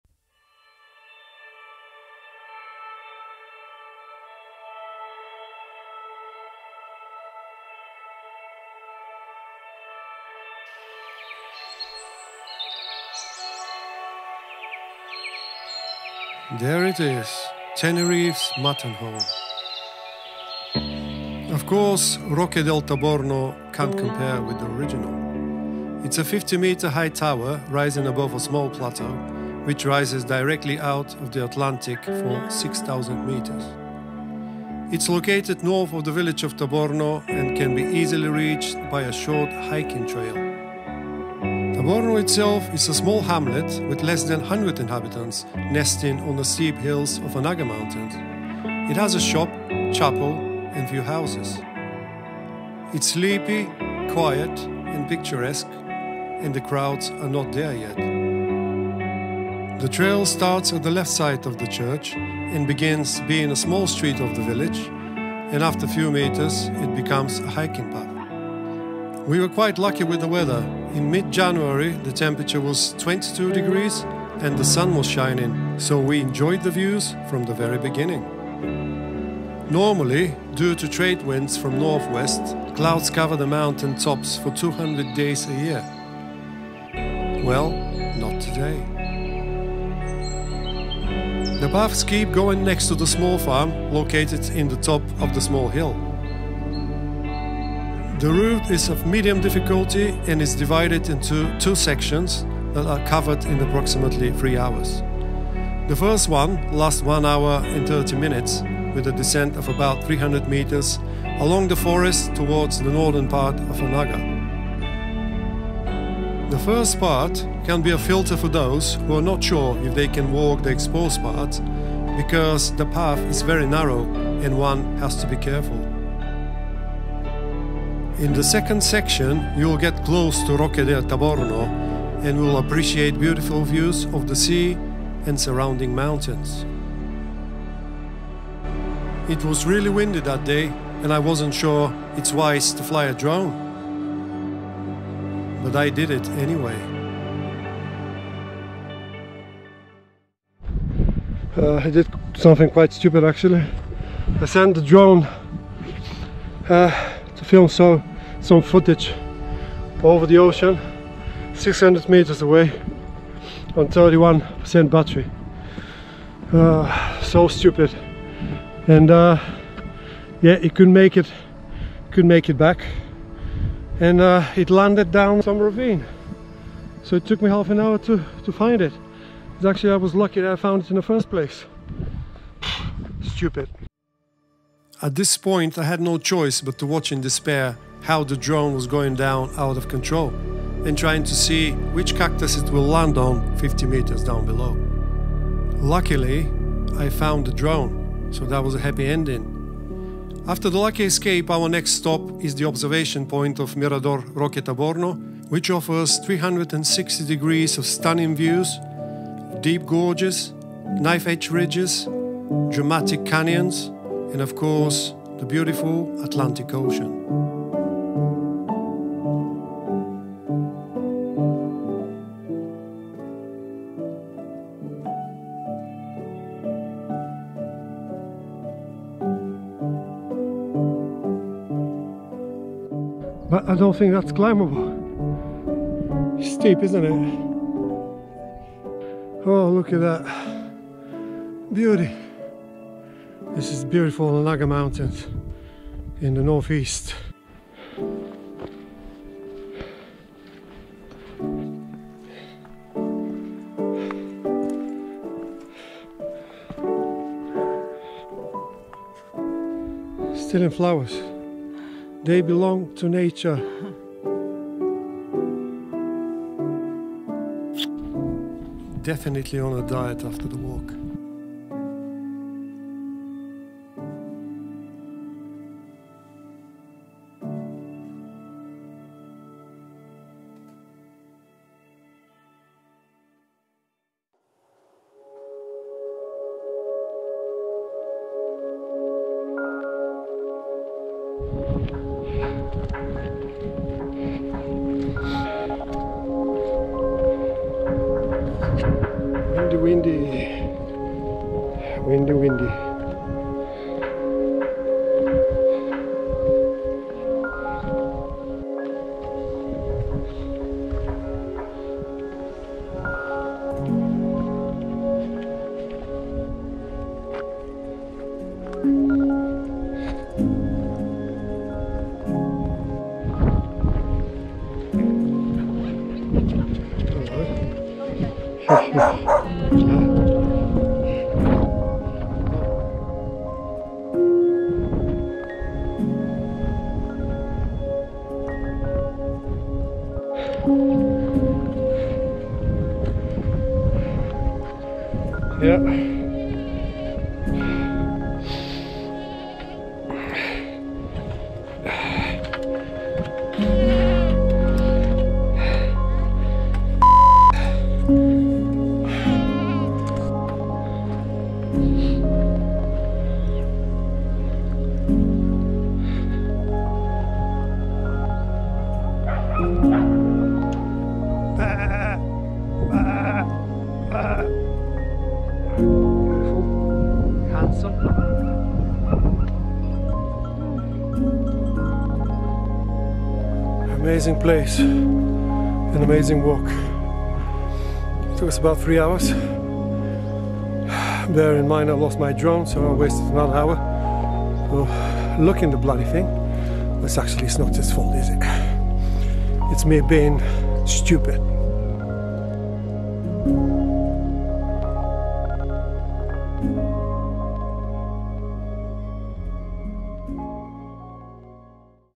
Roque de Taborno Hike in Anaga Mountains | A Drone Lost and Found | Tenerife | 4K
Roque de Taborno hike in Anaga mountains in Tenerife is one of the most beautiful hikes in the area. Although a relatively short one it offers everything you wish for a hike-stunning views, dramatic rock, gorges and canyons and the exciting sense of adventure.